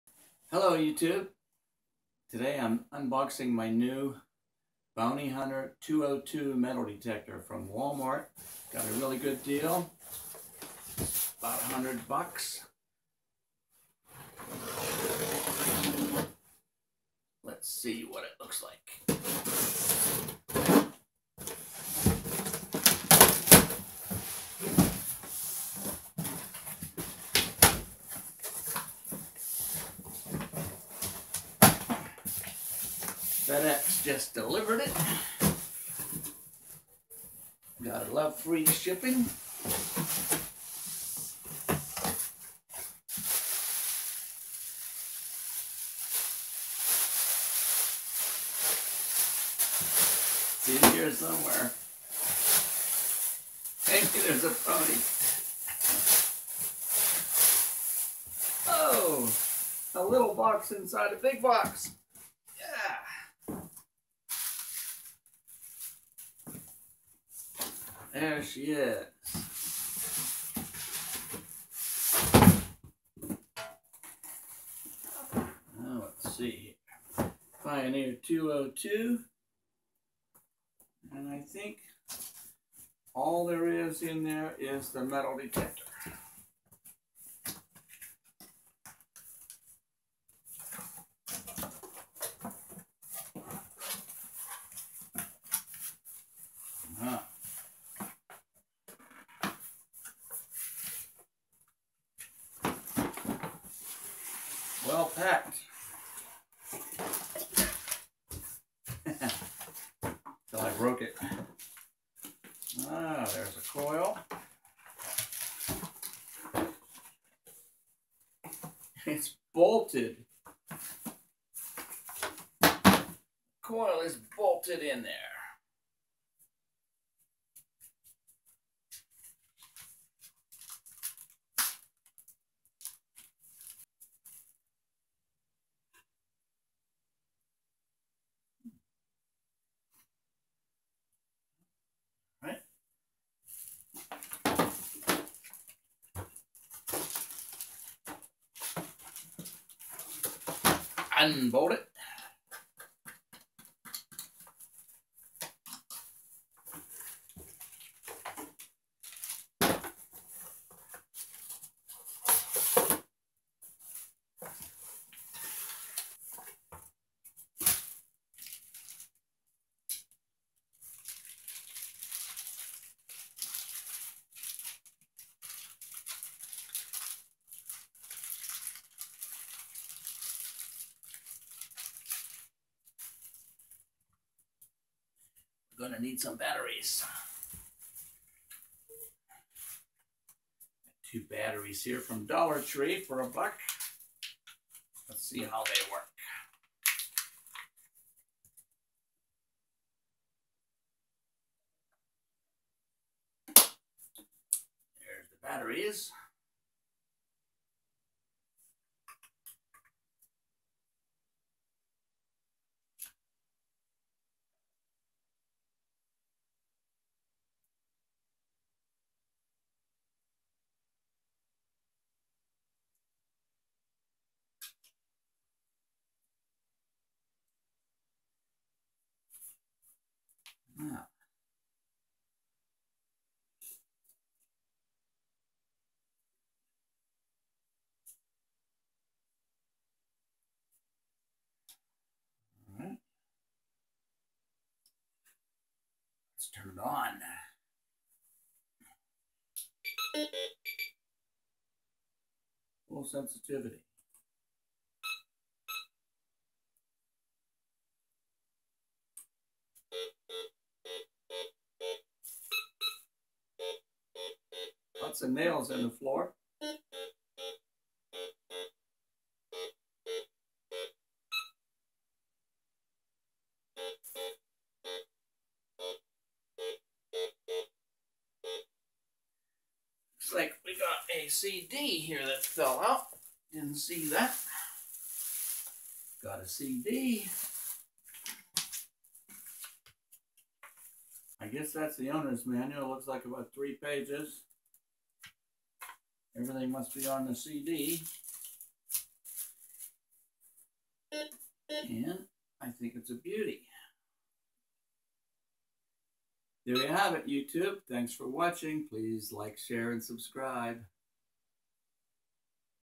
UNBOXING | BOUNTY HUNTER PIONEER 202 | METAL DETECTOR
My new metal detector unboxing and assembly. This seems to be the best bang for buck in my opinion.